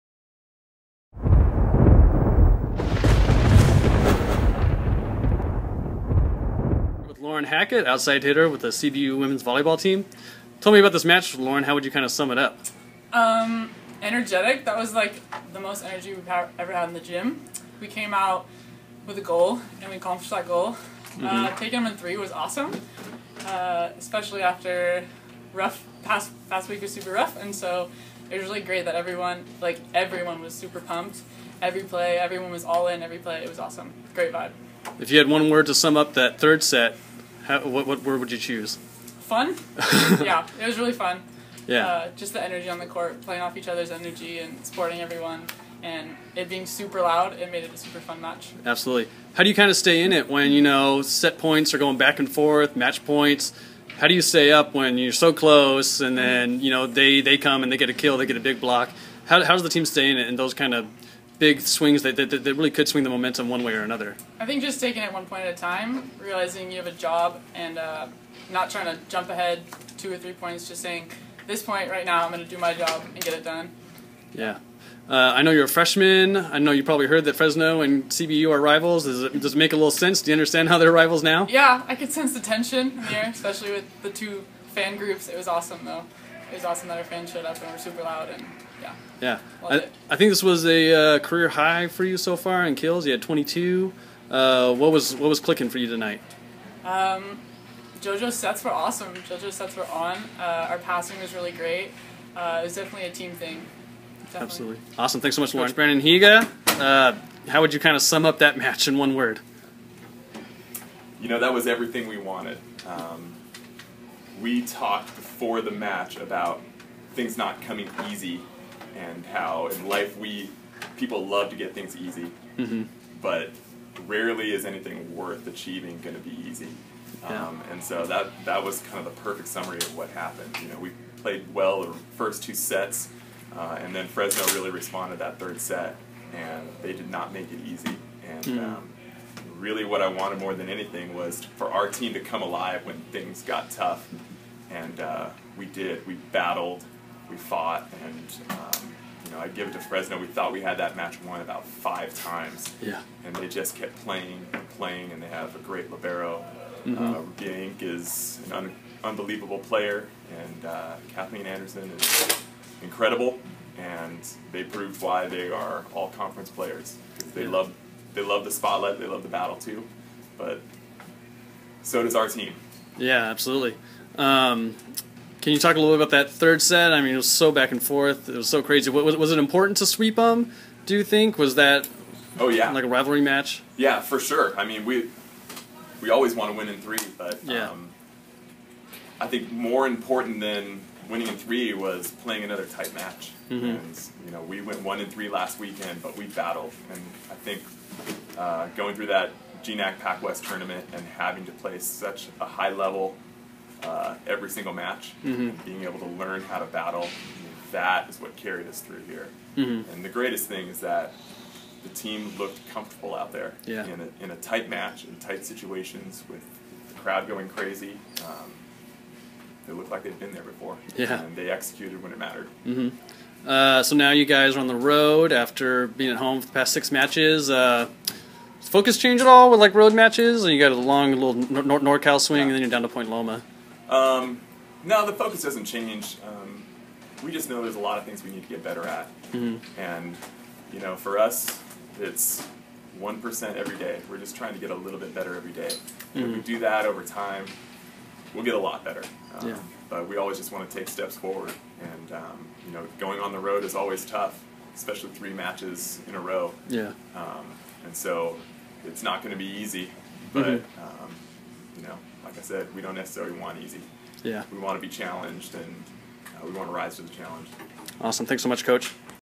Women's Volleyball: CBU vs. Fresno Pacific -- Lauren Hackett and Branden Higa
CBU women's volleyball outside hitter Lauren Hackett and Coach Branden Higa discuss the Lancers sweep of Fresno Pacific.